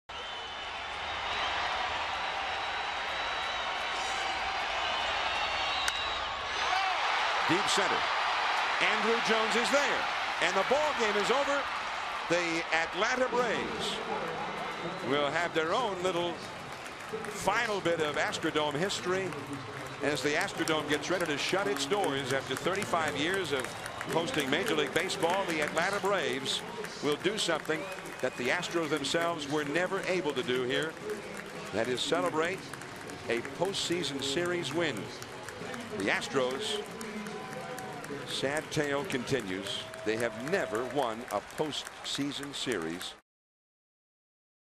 1999 NLDS Gm4: Rocker sends the Braves to NLCS
10/9/99: John Rocker gets Ken Caminiti to fly out to center field as the Braves clinch a trip to the 1999 NLCS